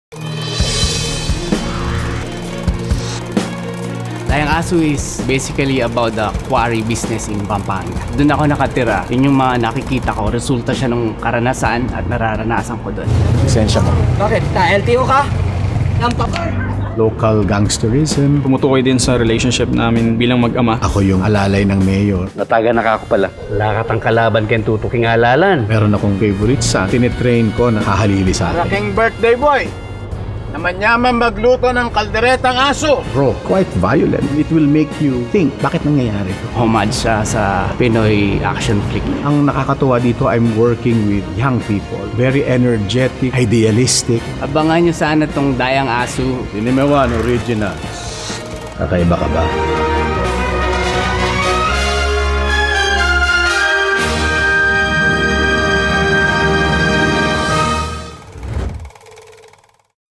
BEHIND THE SCENES - Dayang Asu (C1 Originals 2015 Competition Film)
Directed by Bor Ocampo

Genre: Action-Drama

Amidst legalized corruption, a son struggles to prove to his father that he is fit to survive in a dog-eat-dog world.

Catch "Dayang Asu" in the following schedules:

November 10
7:20 PM, SM Megamall

November 11
2:30 PM, Glorietta

November 12
5:05 PM, SM Megamall
10:00 PM, Glorietta

November 13
9:50 PM, Trinoma

November 14
12:30 PM, SM Megamall
3:15 PM, Resorts World Manila